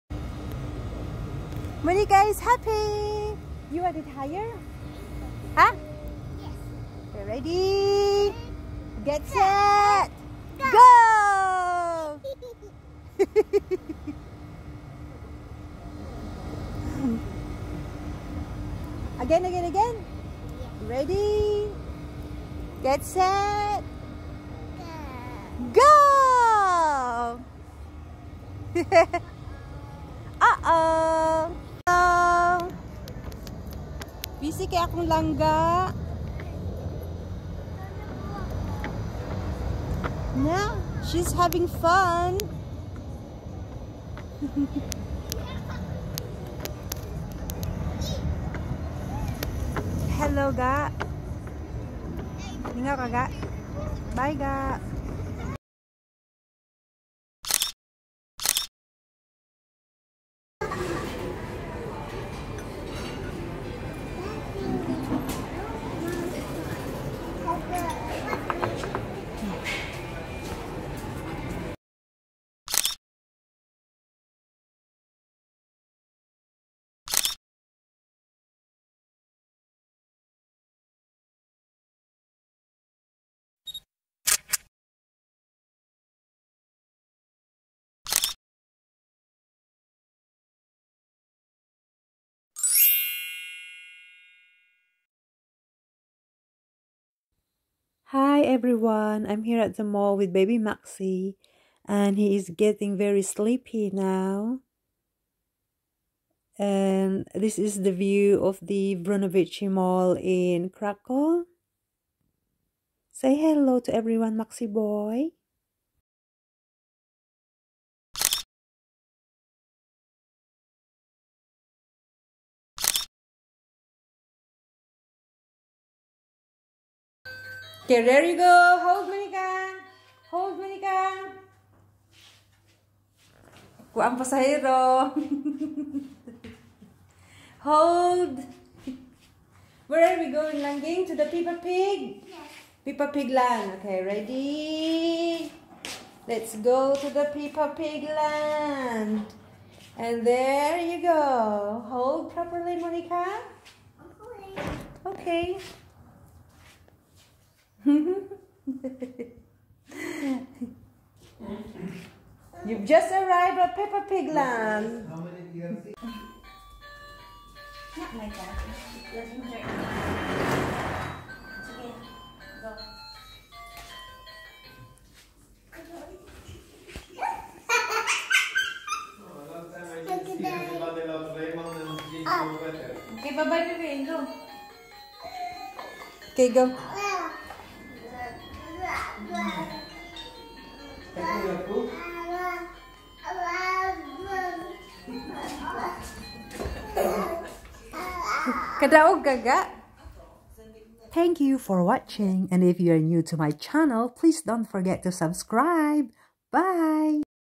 Playground with Monika|MUKBANG@Restaurant|A fun night with Kids
My day with my sister's Family in Poland.